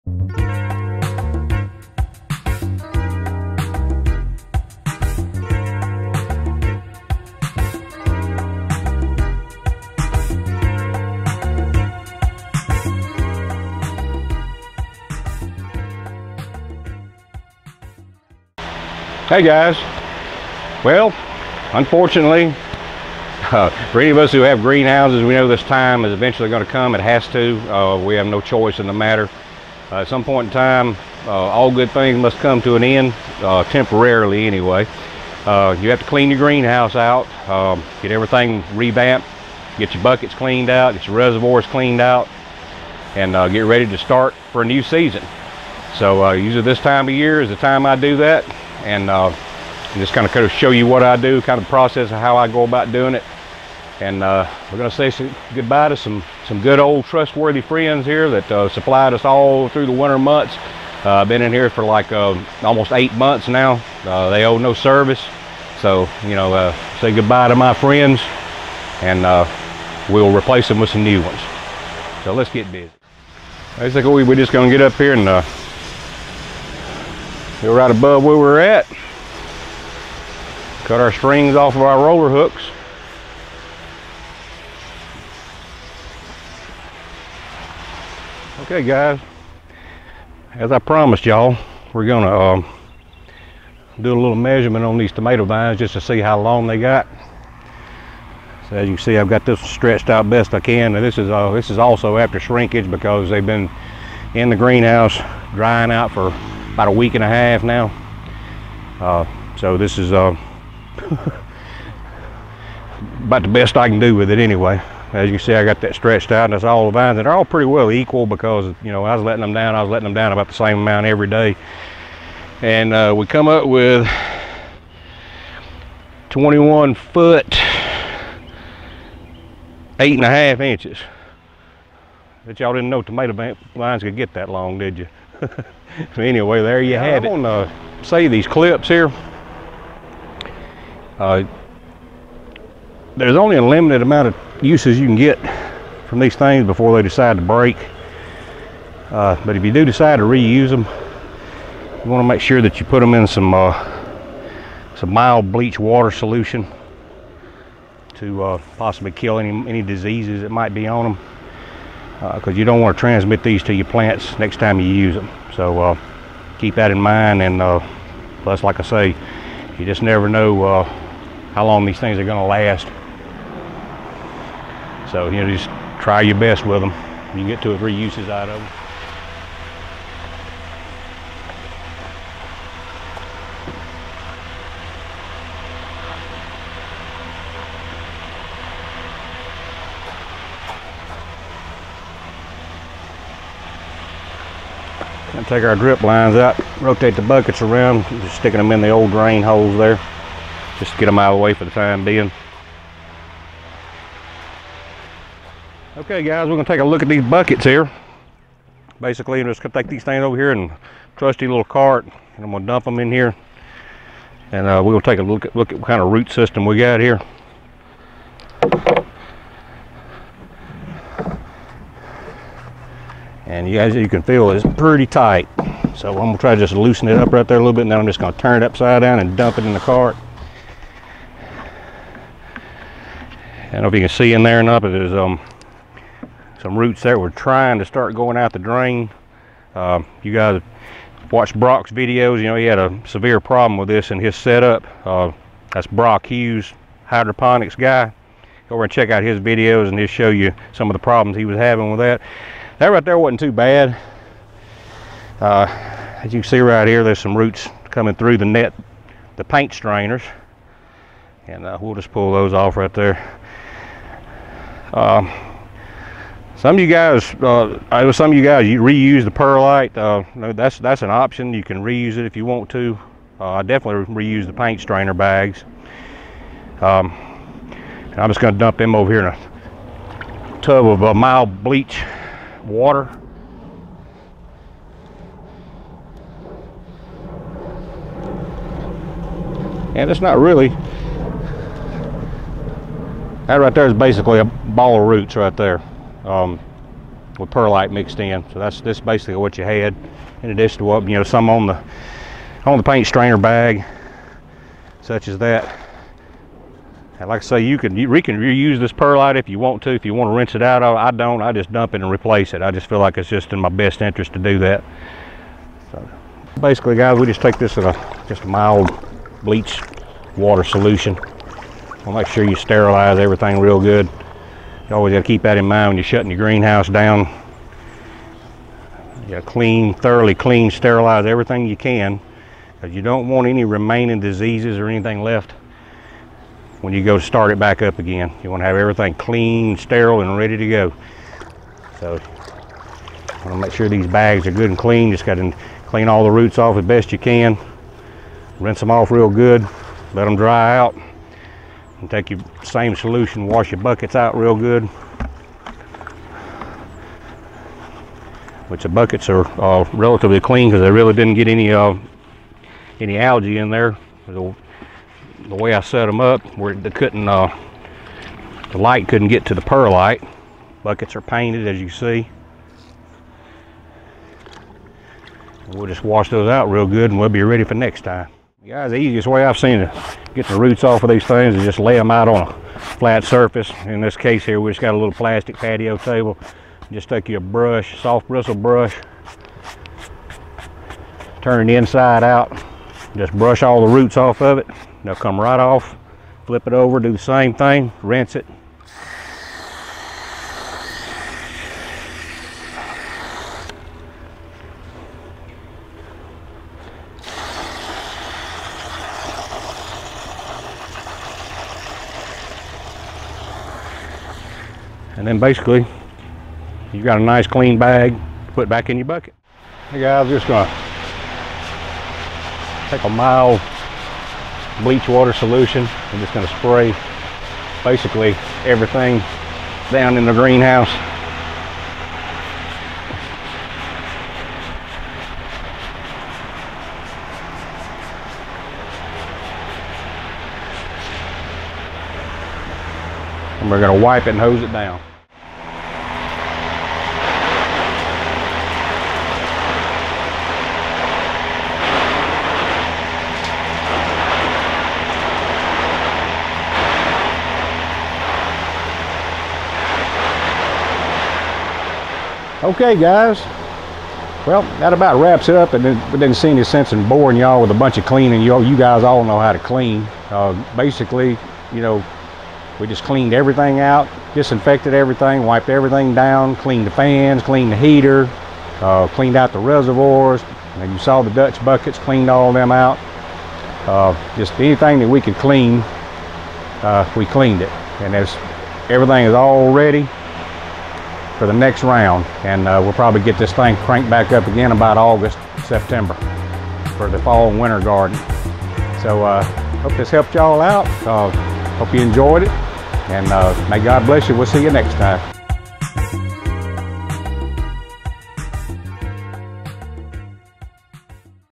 Greenhouse Clean Out With Tomato Vine Measurement
Well, the time has come to say goodbye to some old loyal friends, and good riddance to some unwanted enemies, namely whiteflies! We will go over cleaning the hydroponic greenhouse, dutch buckets, reservoirs, and preparing it for the next go 'round. We will also have some fun measuring the tomato vines!